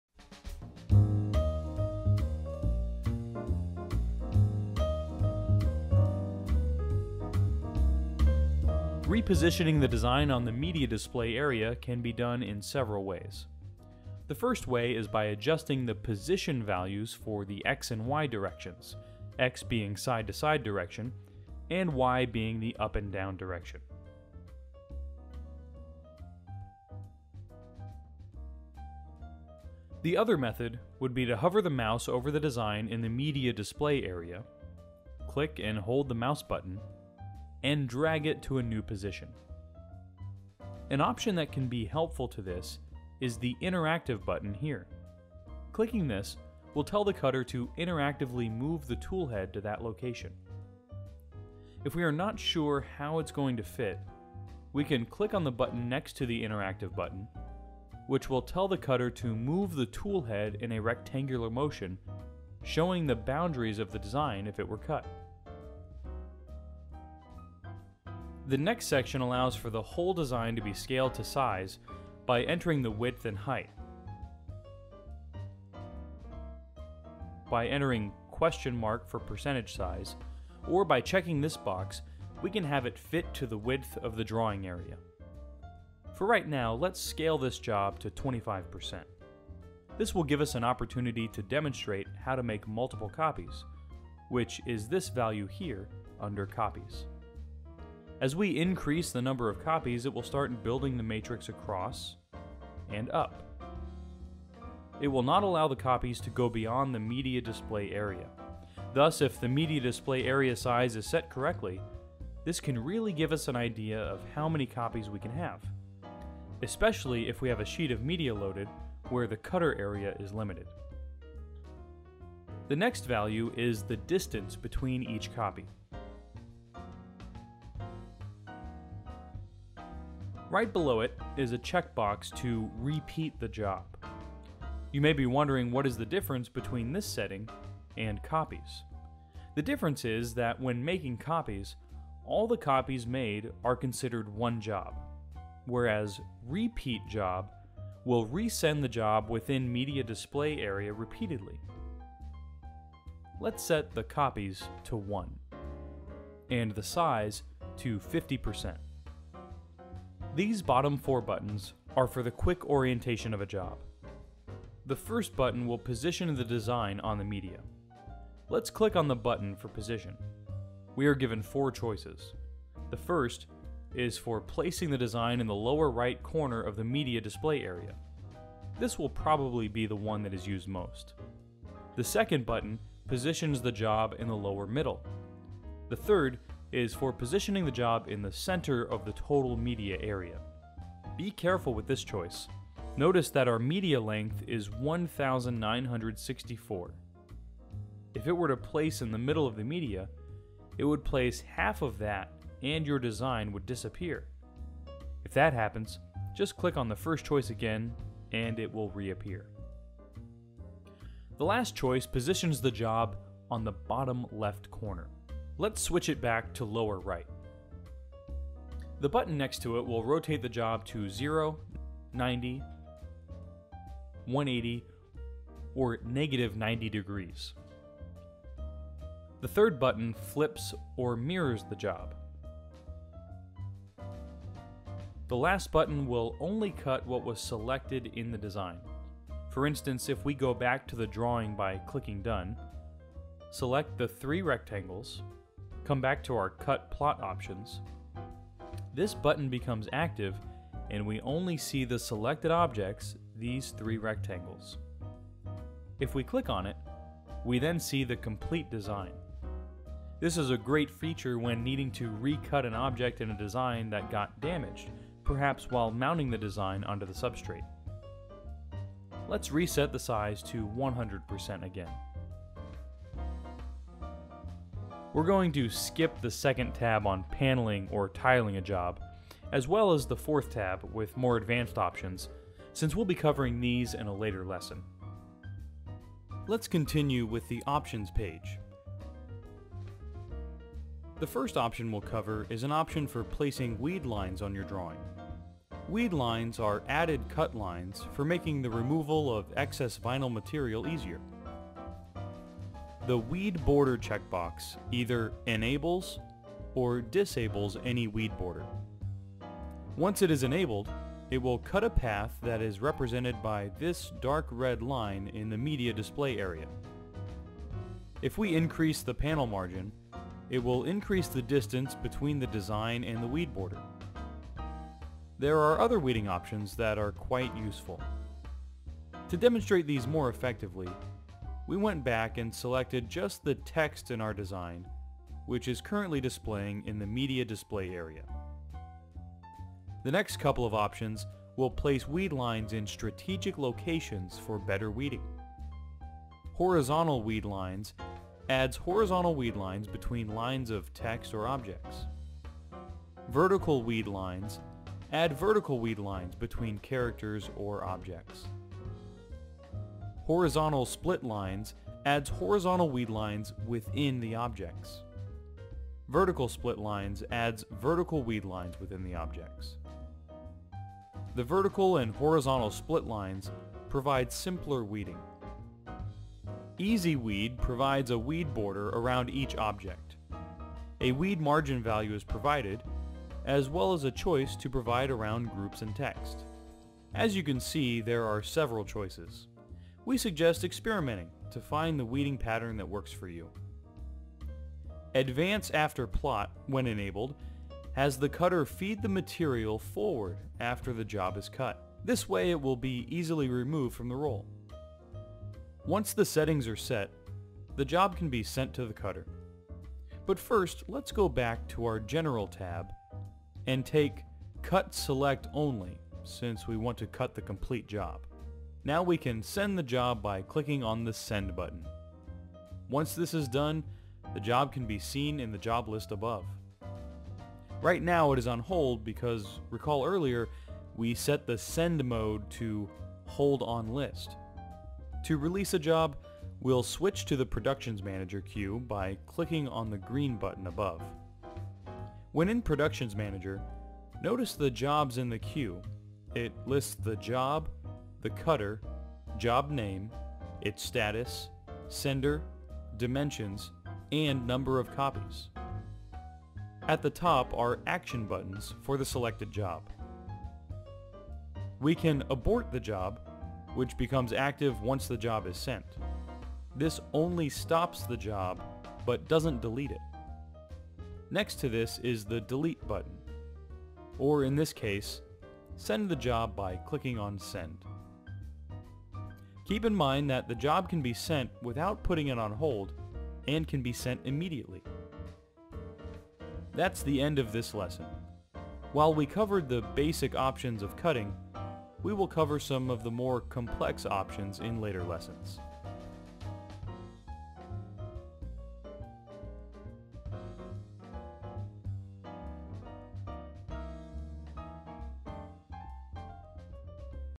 LXi Sign Making Software Cutting a Design Part 2 of 2
Get familiar with LXi Sign Making Software with this helpful video about cutting a design.